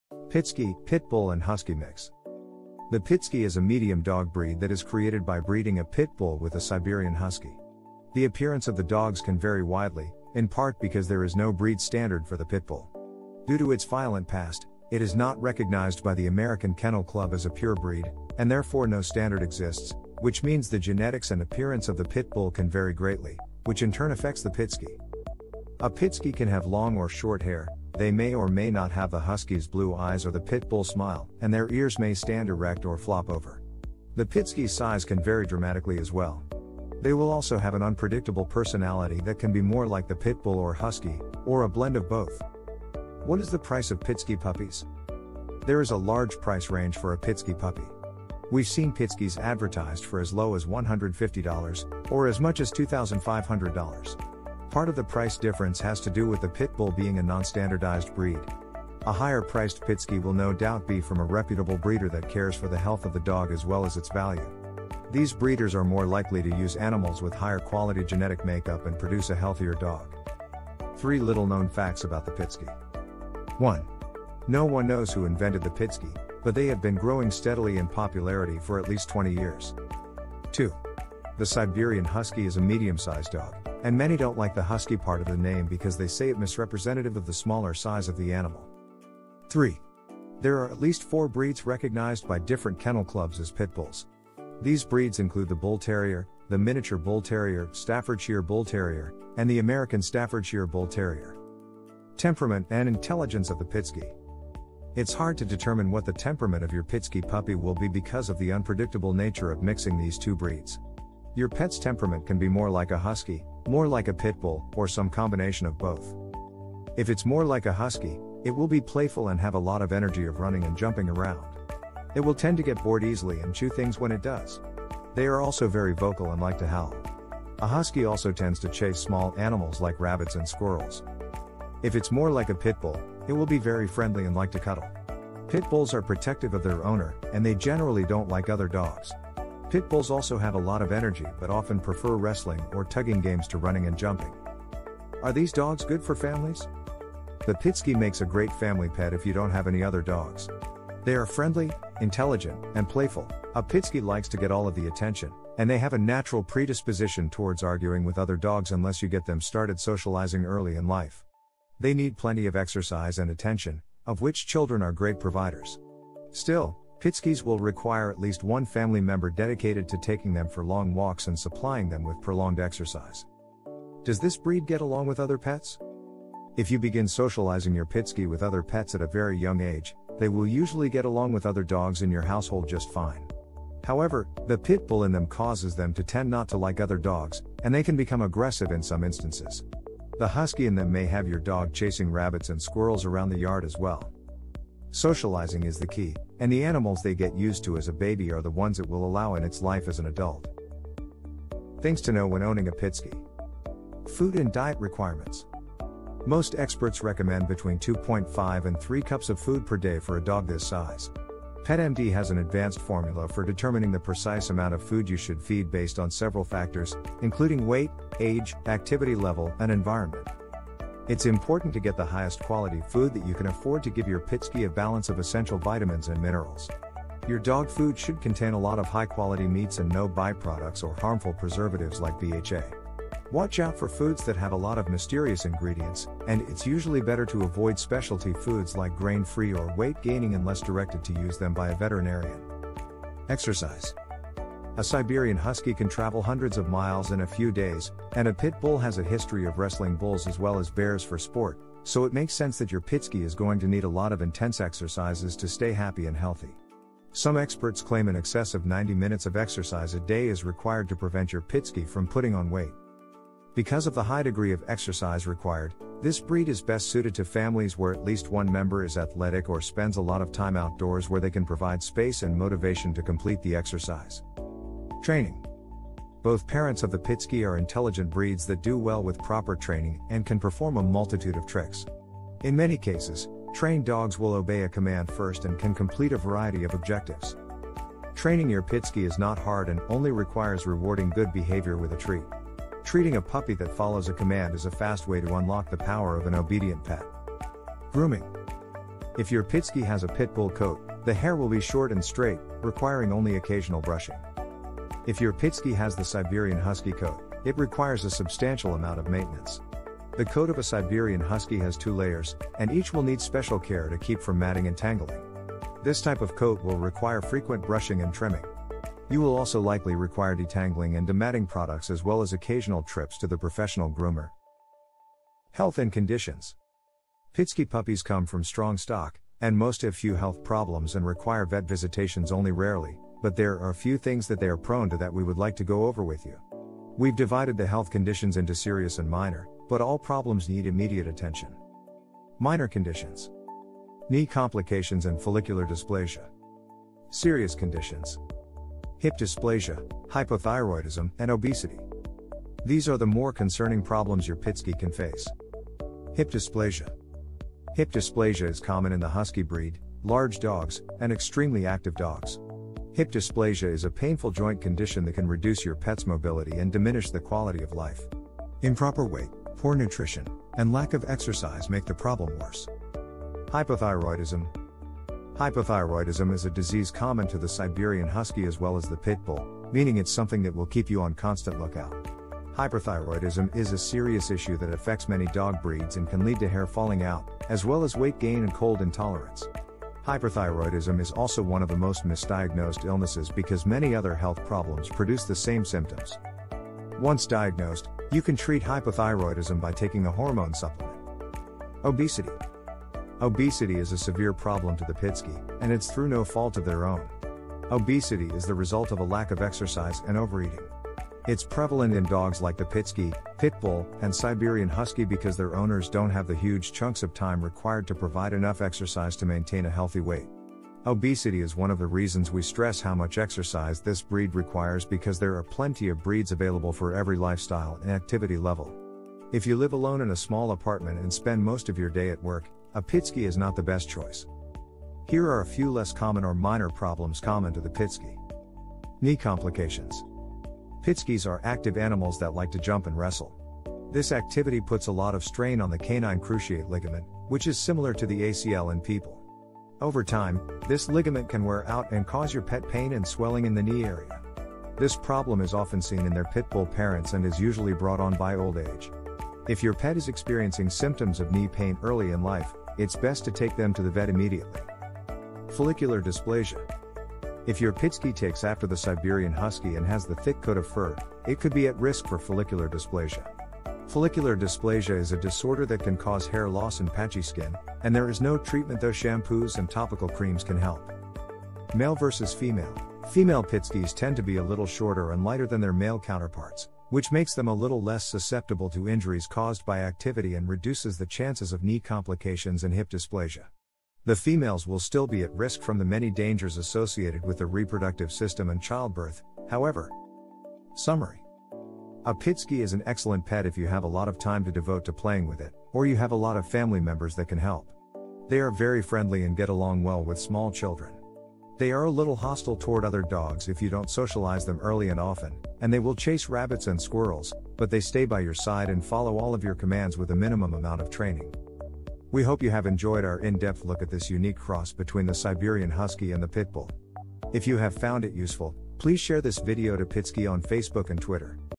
Pitsky (Pitbull & Husky Mix): A Complete Dog Breed Guide
Hi, In this video, we are talking about pitsky (pitbull & husky Mix).

Height: 19 – 24 inches
Weight: 30 – 70 pounds
Lifespan: 12 – 15 years
Colors: Blue, brown, grey, white, red, black
Suitable for: Families, energetic young people
Temperament: Friendly but don’t like competition from other pets

Chapters of this video:

0:00 - Intro
0:43 - What is the Price of Pitsky Puppies?
1:10 - Three Little-Known Facts About the Pitsky
1:41 - Temperament & Intelligence of the Pitsky
2:25 - Are These Dogs Good for Families?
2:54 - Does This Breed Get Along with Other Pets?
3:24 - Food & Diet Requirements
4:02 - Exercise
4:33 - Training
4:57 - Grooming
5:25 - Health and Conditions
8:01 - Male vs Female
8:21 - Summary